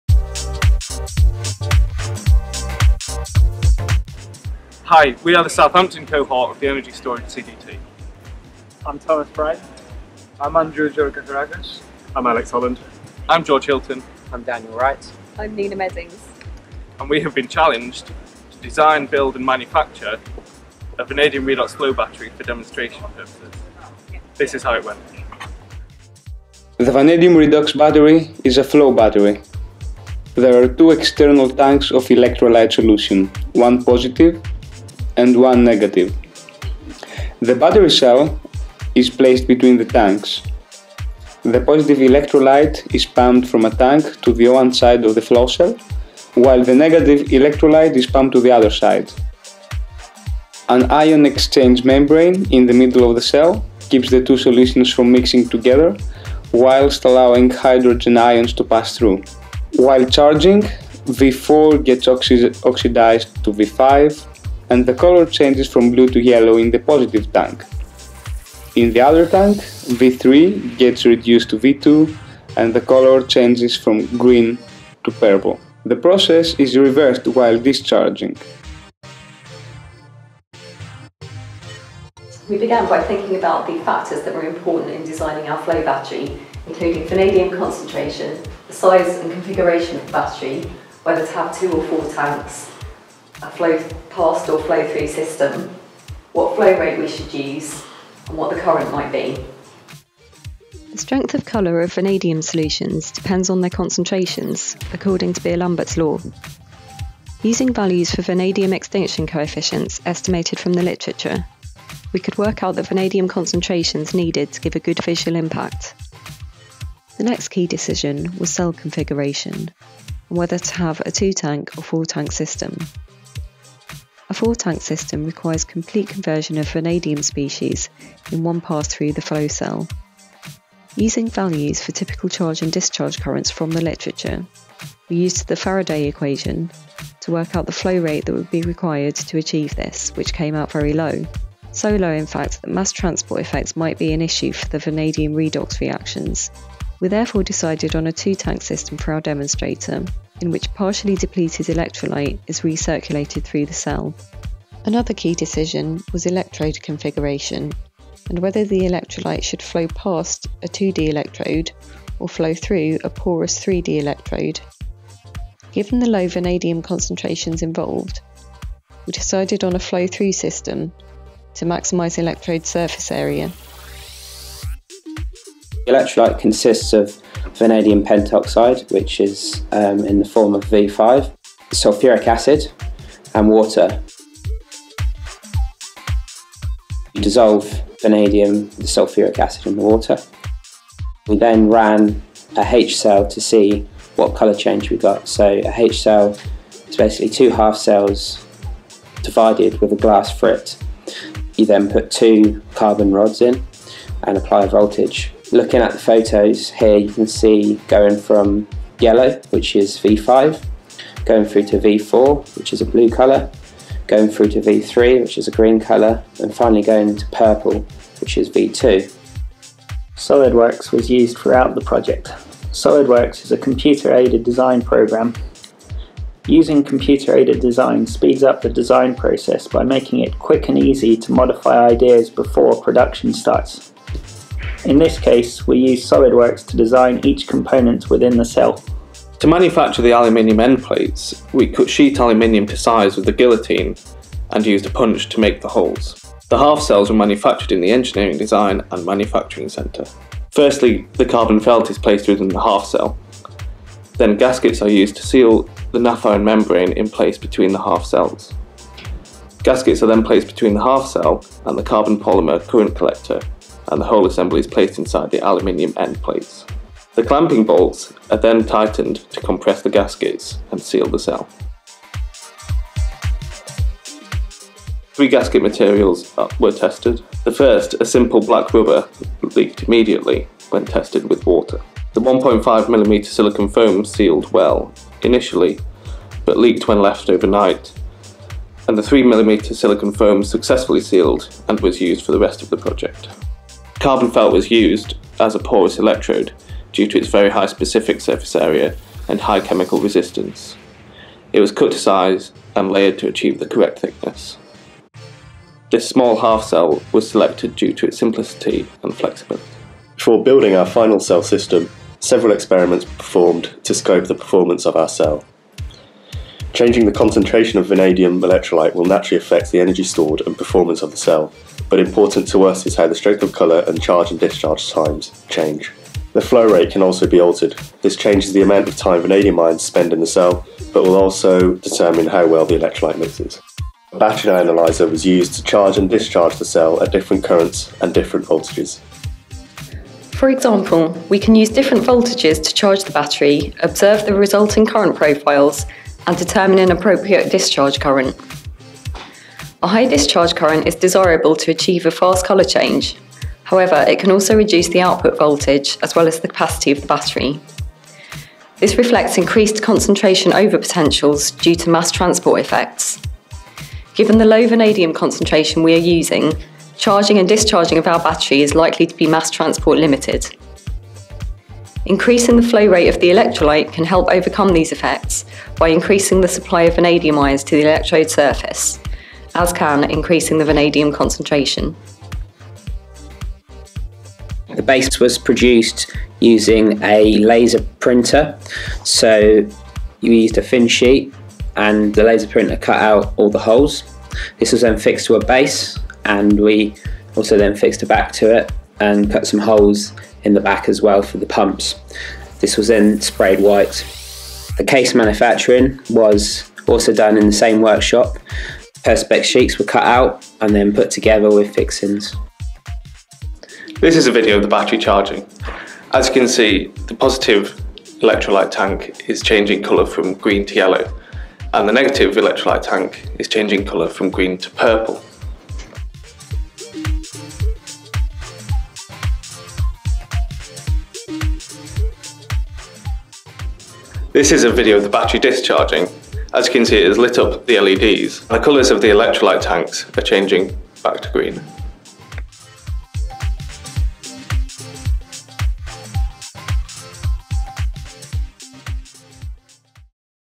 Making a vanadium redox flow battery
The students in the Energy Storage Centre for Doctoral Training at the University of Southampton were challenged to make a battery and test it. Here's what they did.